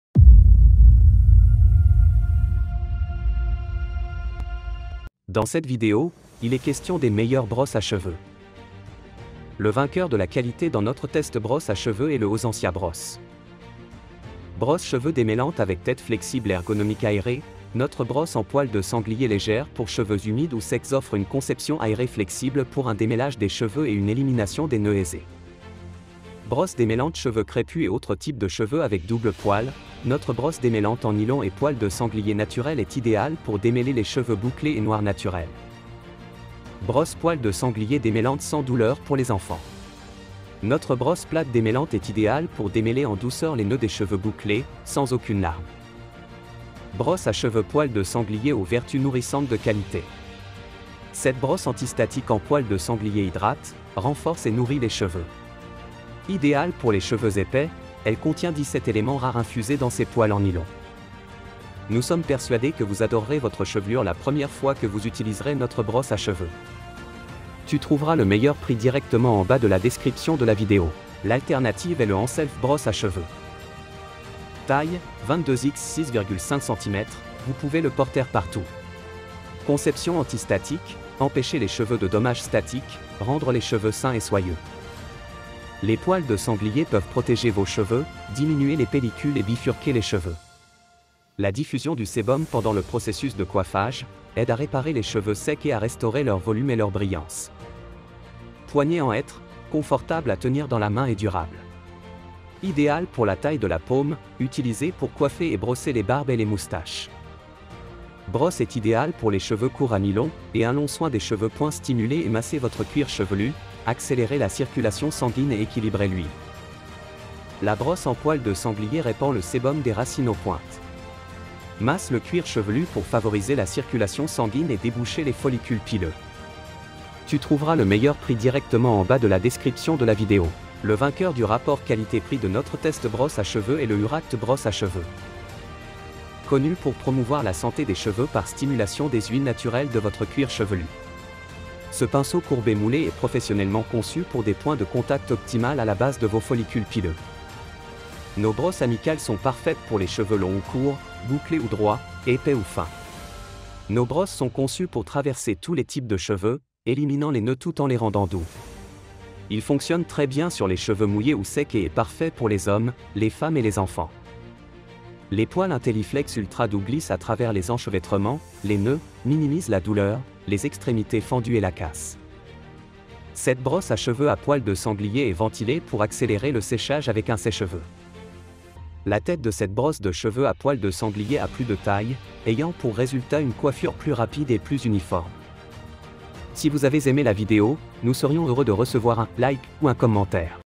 TOP 3 » Meilleur Brosse à Cheveux Poils de Sanglier 2022 ⊕ Le meilleur
Dans cette vidéo, il est question des meilleurs Brosse à Cheveux.

Le vainqueur de la qualité dans notre test Brosse à Cheveux est le Osensia Brosse.

Brosse cheveux démêlante avec tête flexible ergonomique aérée - Notre brosse en poils de sanglier légère pour cheveux humides ou secs offre une conception aérée flexible pour un démêlage des cheveux et une élimination des nœuds aisés.
Brosse démêlante cheveux crépus et autres types de cheveux avec doubles poils - Notre brosse démêlante en nylon et poils de sanglier naturels est idéale pour démêler les cheveux bouclés et noirs naturels.
Brosse poil de sanglier démêlante sans douleur pour les enfants. Notre brosse plate démêlante est idéale pour démêler en douceur les nœuds des cheveux bouclés, sans aucune larme.
Brosse à cheveux poils de sanglier aux vertus nourrissantes de qualité. Cette brosse antistatique en poils de sanglier hydrate, renforce et nourrit les cheveux. Idéale pour les cheveux épais, elle contient 17 éléments rares infusés dans ses poils en nylon.
Nous sommes persuadés que vous adorerez votre chevelure la première fois que vous utiliserez notre brosse à cheveux.
Tu trouveras le meilleur prix directement en bas de la description de la vidéo.

L'alternative est le Anself Brosse a Cheveux.

Taille: 22 x 6,5 centimetres, vous pouvez le porter partout. Conception antistatique, empêcher les cheveux de dommages statiques, rendre les cheveux sains et soyeux.
Les poils de sanglier peuvent protéger vos cheveux, diminuer les pellicules et bifurquer les cheveux. La diffusion du sébum pendant le processus de coiffage, aide à réparer les cheveux secs et à restaurer leur volume et leur brillance.
Poignée en hêtre, confortable à tenir dans la main et durable. Idéal pour la taille de la paume, utilisé pour coiffer et brosser les barbes et les moustaches.
Brosse est idéal pour les cheveux courts à mi-longs, et un long soins des cheveux.Stimulez et massez votre cuir chevelu, accélérez la circulation sanguine et équilibrez l'huile.
La brosse en poils de sanglier répand le sébum des racines aux pointes. Masse le cuir chevelu pour favoriser la circulation sanguine et déboucher les follicules pileux.
Le vainqueur du rapport qualité-prix de notre test Brosse à Cheveux est le Uraqt Brosse a Cheveux.

Connu pour promouvoir la santé des cheveux par stimulation des huiles naturelles de votre cuir chevelu. Ce pinceau courbé moulé est professionnellement conçu pour des points de contact optimales à la base de vos follicules pileux.
Nos brosses amicales sont parfaites pour les cheveux longs ou courts, bouclés ou droits, épais ou fins. Nos brosses sont conçues pour traverser tous les types de cheveux, éliminant les nœuds tout en les rendant doux. Il fonctionne très bien sur les cheveux mouillés ou secs et est parfait pour les hommes, les femmes et les enfants.
Les poils Intelliflex ultra doux glissent à travers les enchevêtrements, les nœuds, minimisent la douleur, les extrémités fendues et la casse.
Cette brosse à cheveux à poils de sanglier est ventilée pour accélérer le séchage avec un sèche-cheveux.
La tête de cette brosse de cheveux à poils de sanglier a plus de taille, ayant pour résultat une coiffure plus rapide et plus uniforme.